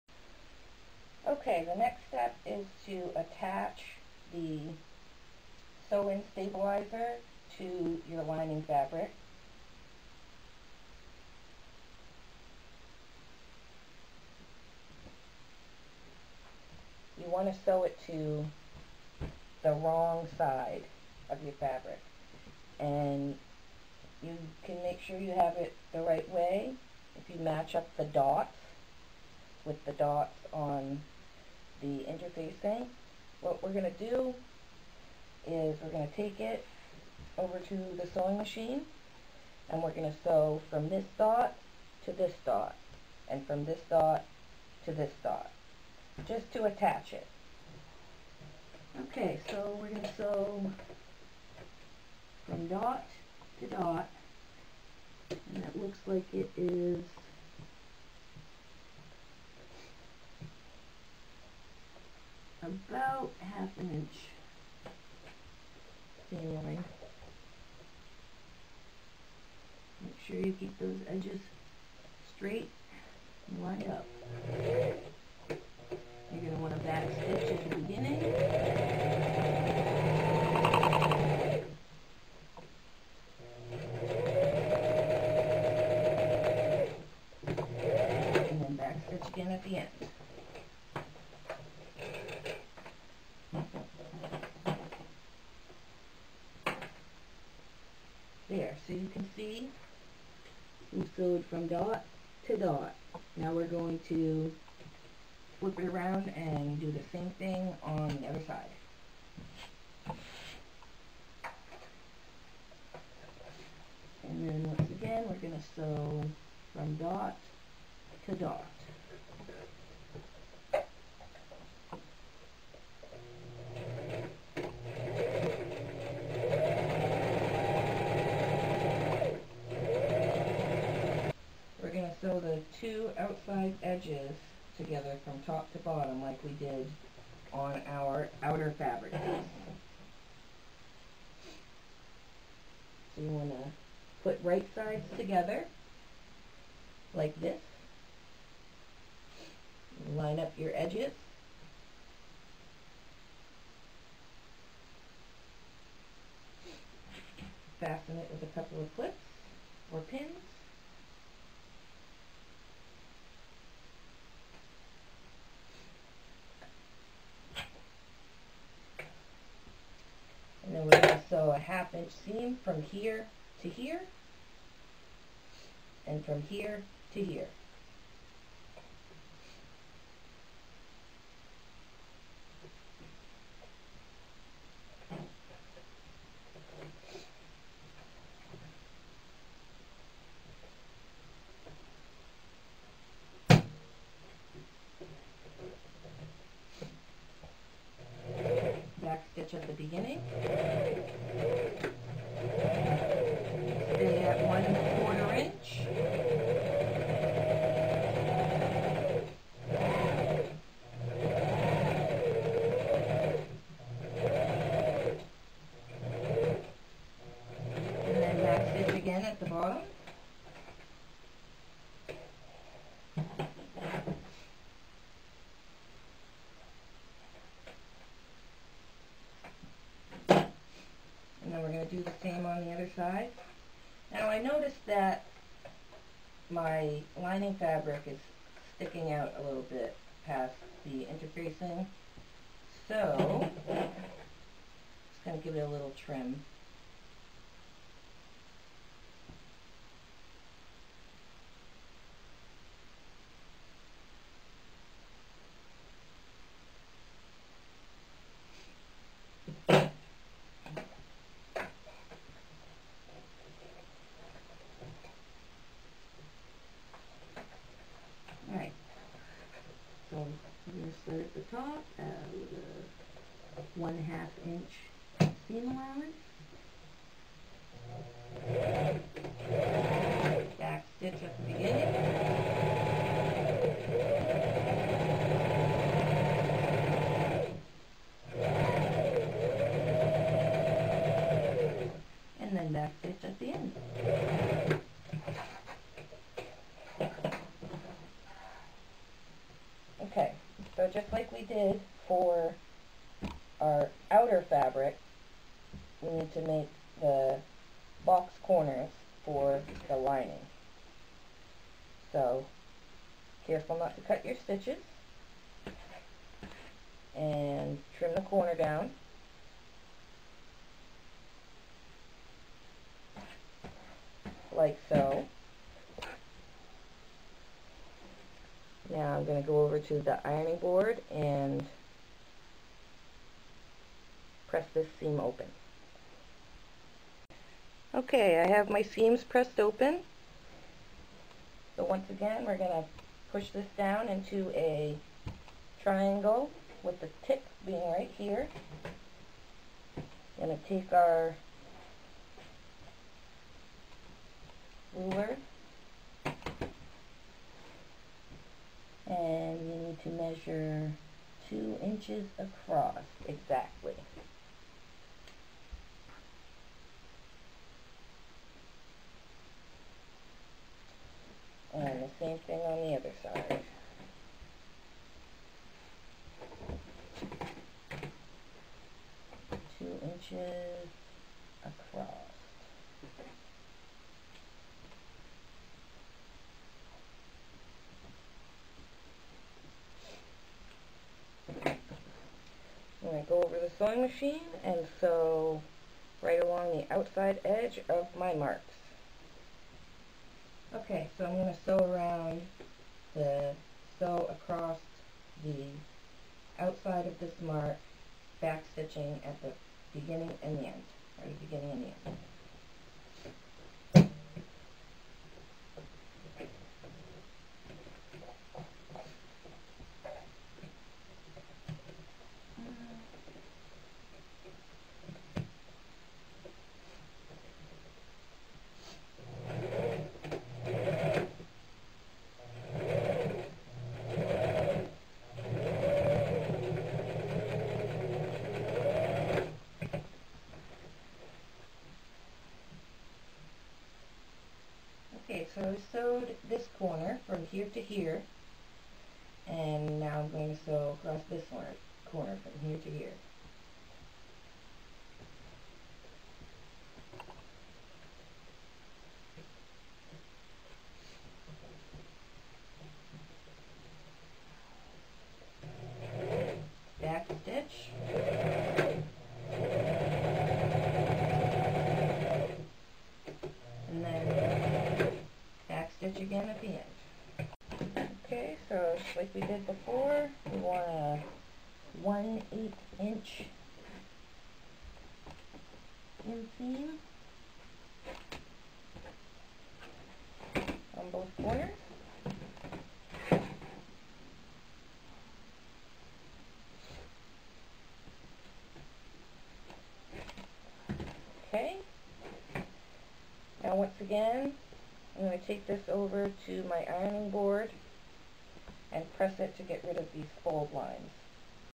Modern Clutch from Burlap Coffee Sack Part 5
I love finding new ways to up-cycle burlap coffee sacks. A local coffee company gave me a ton of these a while back, and I still have plenty left to use up. For this project I used the Modern Clutch pattern from Upstyle. I made a few changes to accommodate to use of burlap as the main bag fabric. After completing the bag I felt the bag looked a bit plain, so I stenciled some flowers on the front. This was a fun intermediate sewing project.